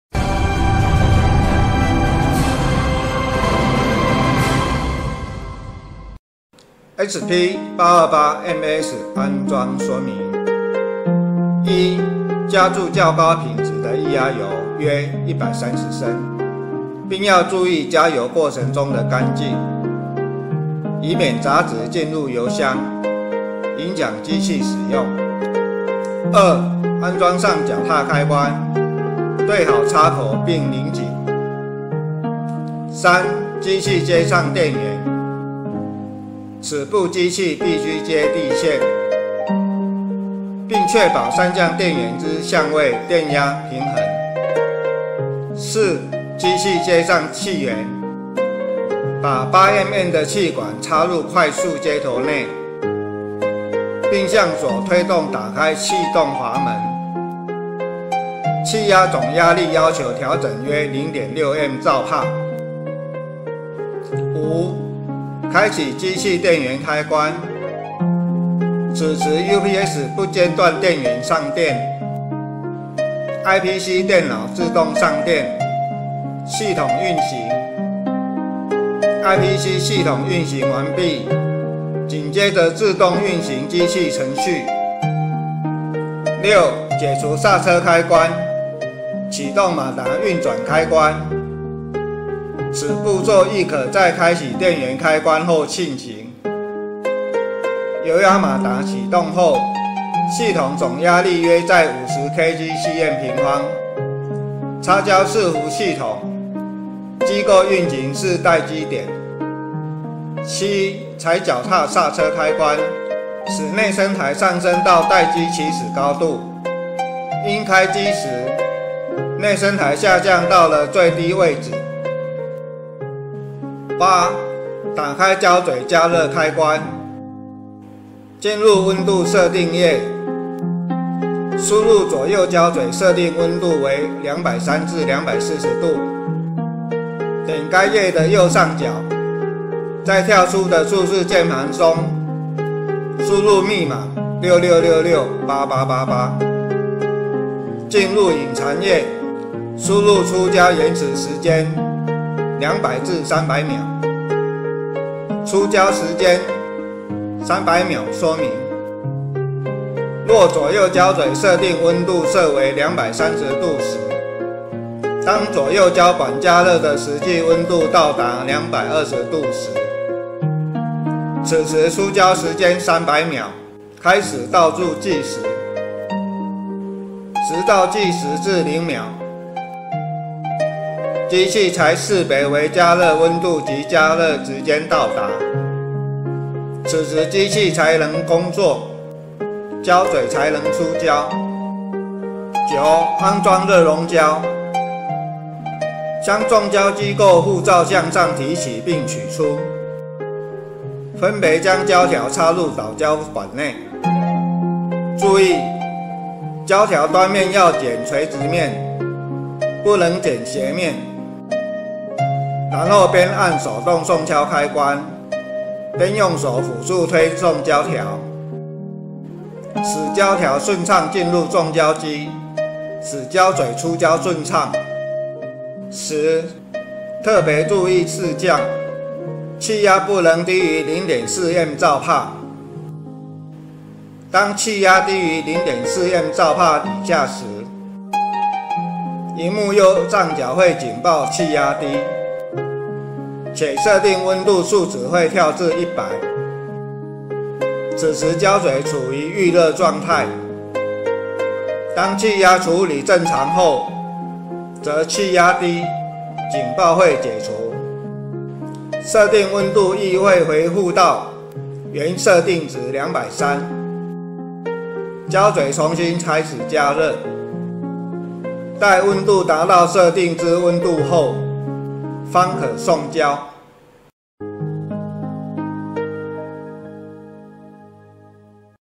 SP-828MAS- Installation 安裝教學
0:12 Fill up hydraulic oil 加入液壓油
0:26 foot switch installation 安裝腳踏開關
0:33 Power supply connection 接電教學
0:46 Pneumatic power supply 氣壓接管
1:04 Machine power switch 啟動機器
1:26 Starting the machine & checking points 開機及各檢查點
1:51 Supporter position resume 內撐台位置
2:03 Tracer's temperature setting 膠筆溫度設定
3:14 Cement installation 熱熔膠安裝
3:46 Pneumatic pressure setting 氣壓壓力調整